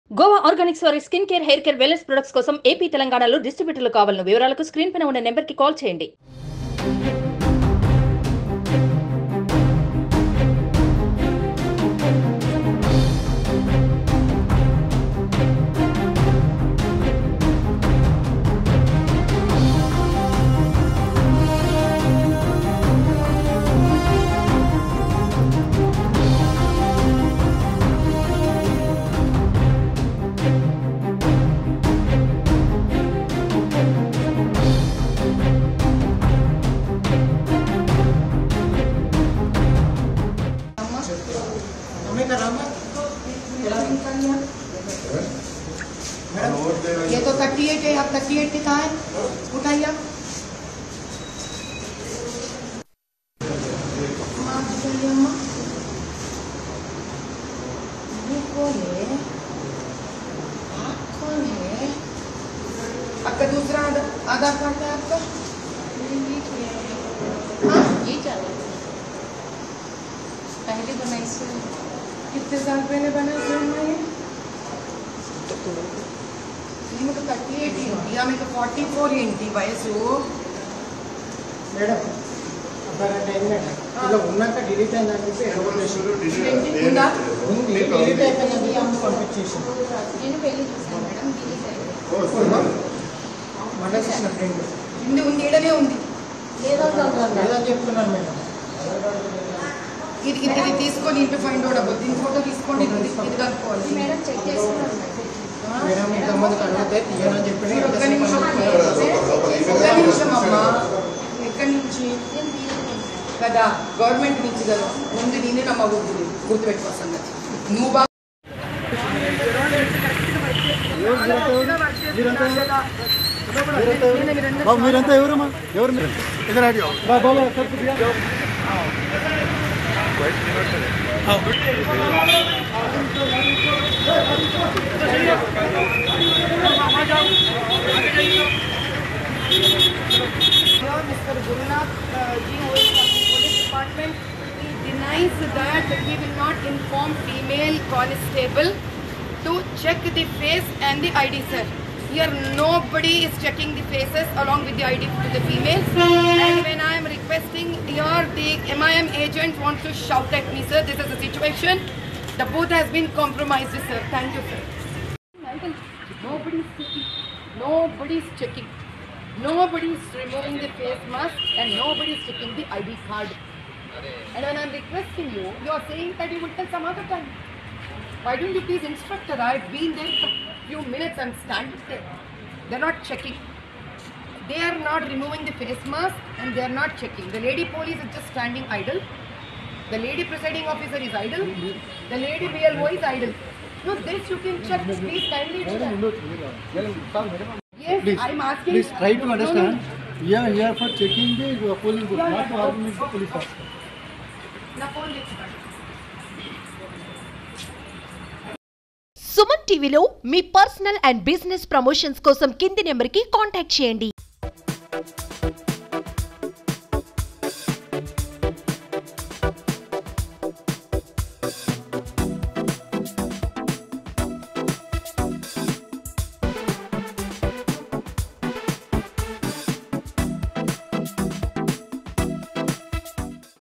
Case Filed Against Madhavi Latha for Checking Muslim Woman ID Card inside Polling Booth | Sumantv
Watch► Case Filed Against Madhavi Latha for Checking Muslim Woman ID Card inside Polling Booth @SumanTV